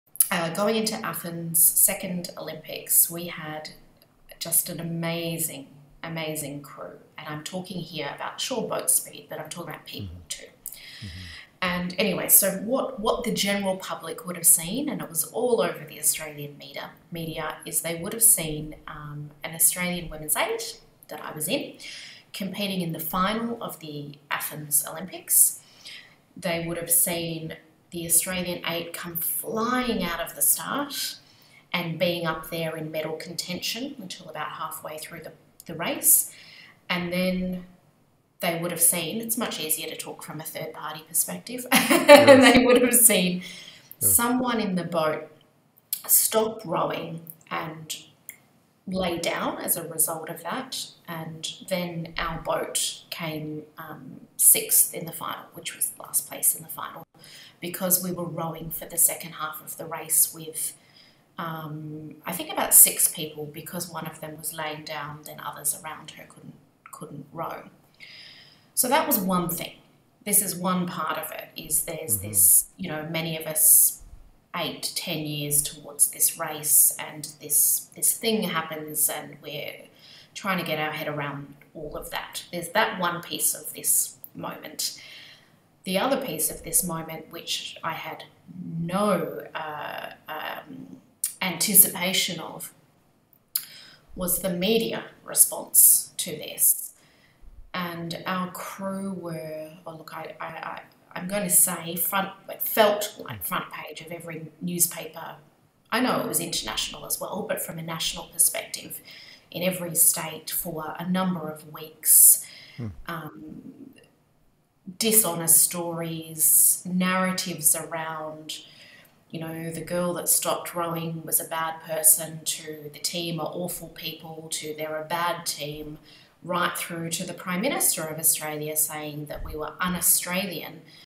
Katie Foulkes Recounts Her Olympic Crucible in 2004
Australian rowing team member Katie Foulkes wasn’t prepared for the firestorm that came when one of her teammates stopped rowing during an Olympic race in 2004.

Katie Foulkes had to harness resilience as a member of Australia’s Olympic rowing team in 2004. After one of her teammates stopped rowing during the race, the outrage that erupted (the country’s prime minister called the team “UnAustralian”) rocked her to her core. Now a leadership coach who researches what builds resilience, she’s found it’s about more than just digging deep within yourself. It also requires casting wide outside yourself — calling on the resources around you to help you survive your crucible and thrive beyond it.

To learn more about Katie Foulkes, her research and her leadership coaching practice,